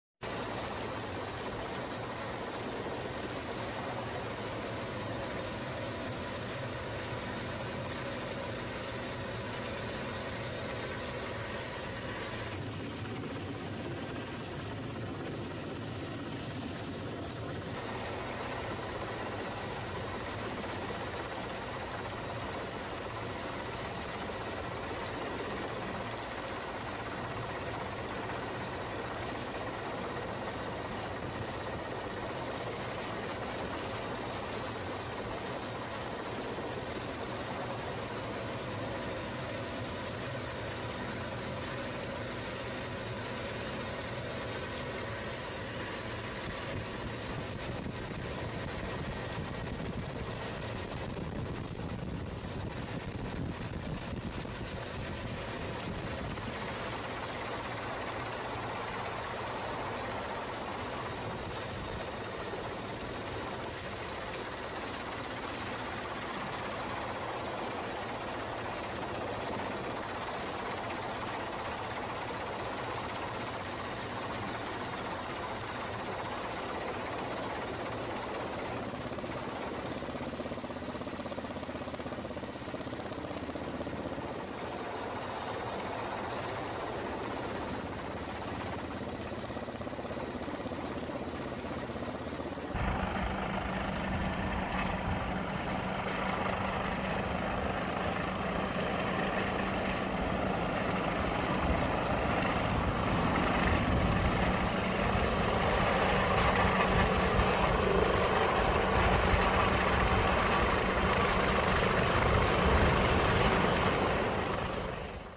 Ка-62 совершил первый полет в Приморье
На летно-испытательной станции Арсеньевской авиационной компании «Прогресс» им. Н.И. Сазыкина холдинга «Вертолеты России» летный образец новейшего вертолета Ка-62 совершил первый испытательный полет.